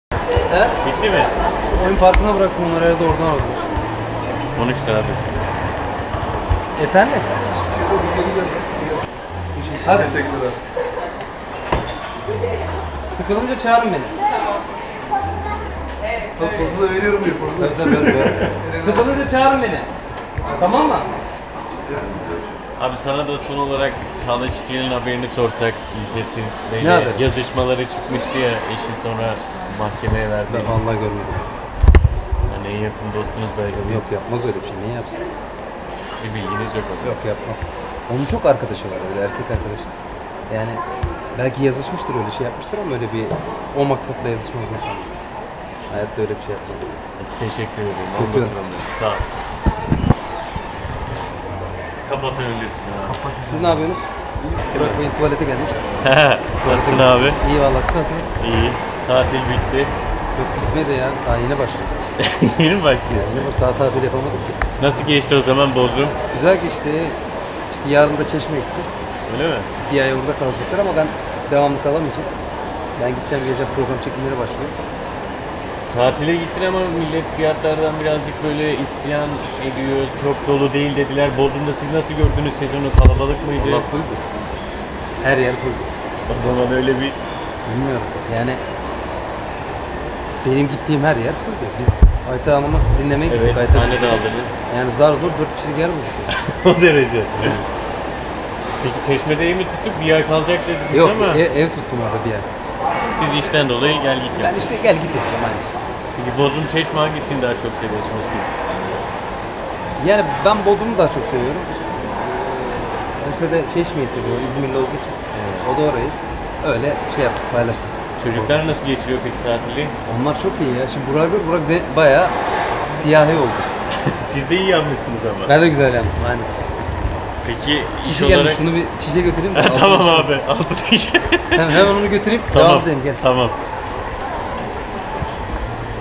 Alişan, Çağla Şıkel'i Savundu, Çağla'nın Çok Erkek Arkadaşı Var
Türkiye’nin yeni YÜZ’ü Nar Channel son dakika haberleri, magazin haberleri ve magazine ait tüm gelişmelerle karşınızda.